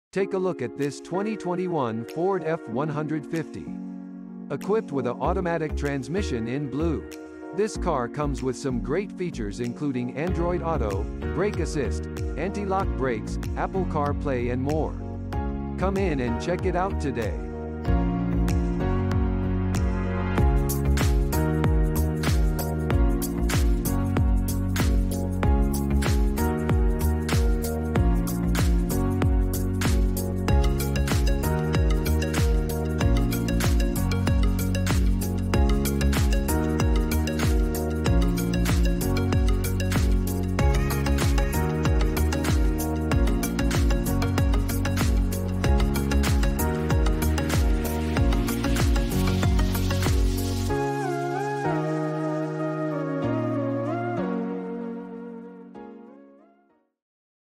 2021 Ford F-150 XLT FTP3154
Take a look at this 2021 Ford F-150! Equipped with a Automatic transmission in Blue. This car comes with some great features including Android Auto, Brake Assist, Antilock Brakes, Apple CarPlay and more. Come in and check it out today!